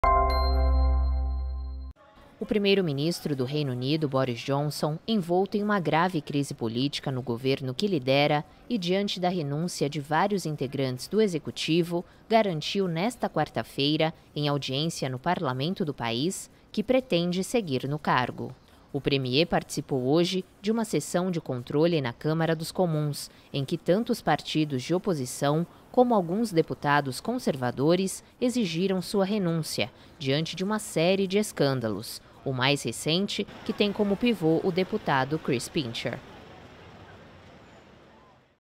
Boris Johnson garante que não entregará cargo de primeiro-ministro
Londres, 6 jul (EFE/EPA).- (Imagens: Clàudia Sacrest/Tolga Akmen) O primeiro-ministro do Reino Unido, Boris Johnson, envolto em uma grave crise política no governo que lidera e diante da renúncia de vários integrantes do Executivo, garantiu nesta quarta-feira, em audiência no parlamento do país, que pretende seguir no cargo.